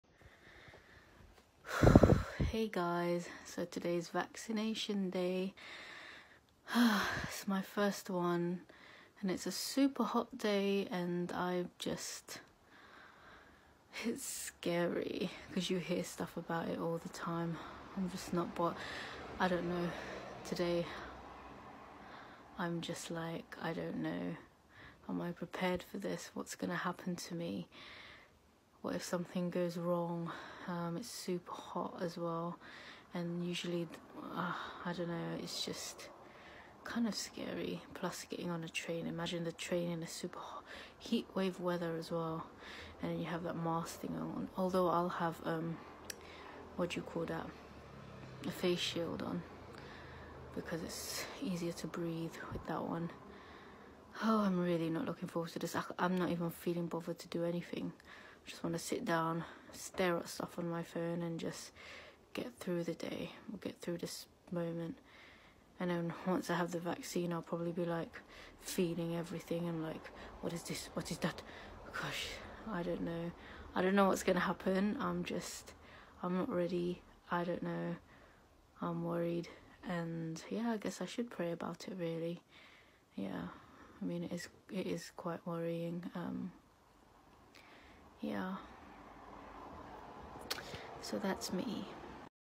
Vaccine day
The first jab, and this is few hours before the first jab. Let’s see what happens - hopefully nothing bad